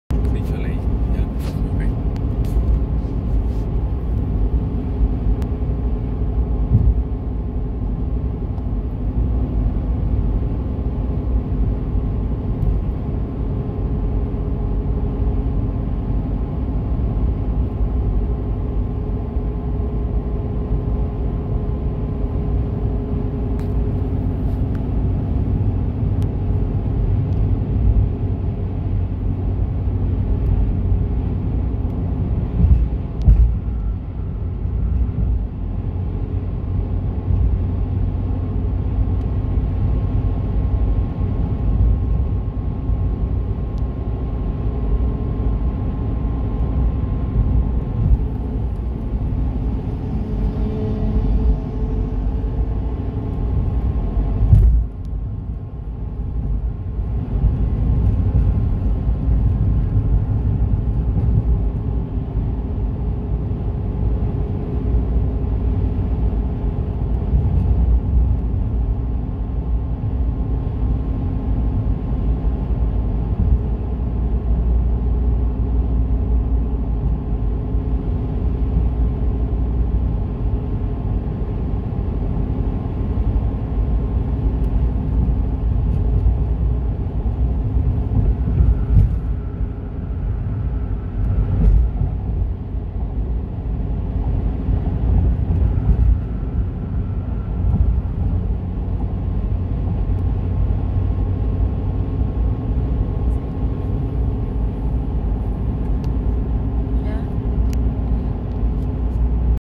un maravilloso atardecer 2-28-21
De regreso a casa y a la vez disfrutando de este maravilloso atardecer.